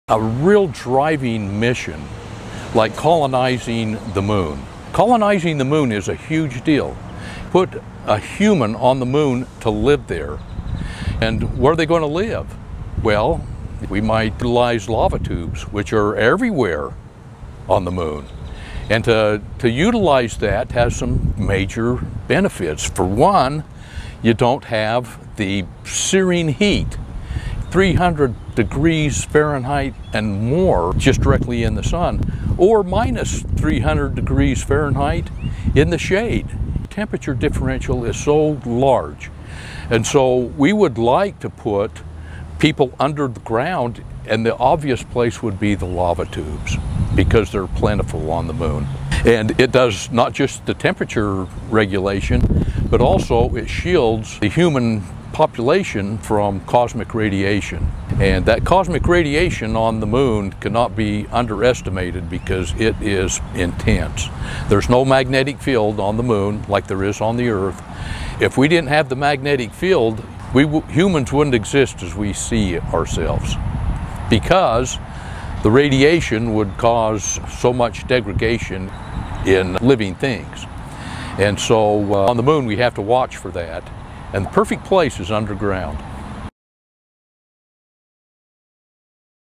How will we live on the Moon?
Where are we thinking of exploring habitats on the Moon? 🌖

Lava Tubes on the Moon can protect people from the harsh radiation and temperature fluctuations caused by the Sun ☀️

When should we start exploring these natural tunnels below the lunar surface? Share what you think 💭

Learn more about our mission at AstroTechnology.com